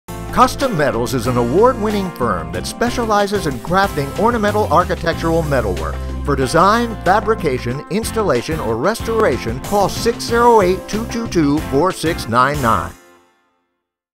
Custom Metals Inc.
"Custom Metals is an award-winning firm that specializes in crafting ornamental architectural metalwork: from railings and staircases, to kitchens, signs, fireplaces furniture and more! Working since 1980 on commercial, residential, and restoration projects, Custom Metals is skilled in a wide range of metals and specialty finishes.